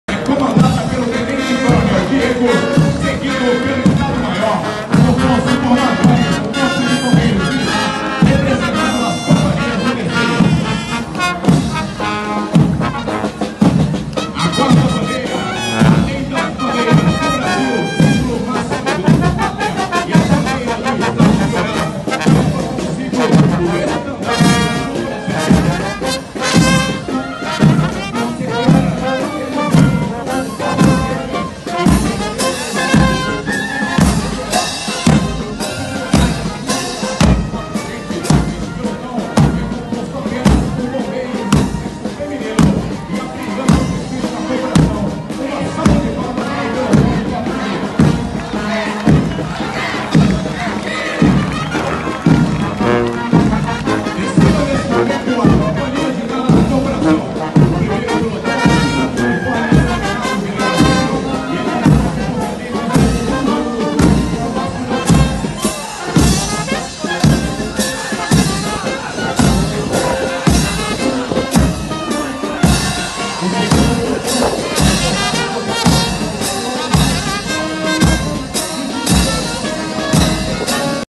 Banda de Música do Corpo de Bombeiros do Estado de Goiás
BANDA DE MÚSICA DO CORPO DE BOMBEIROS MILITAR DO ESTADO DE GOIÁS. Desfile de 7 de setembro 2019.
LOCAL: Av Tocantins, em Goiânia-GO.
Equipe do Portal Brasil Sonoro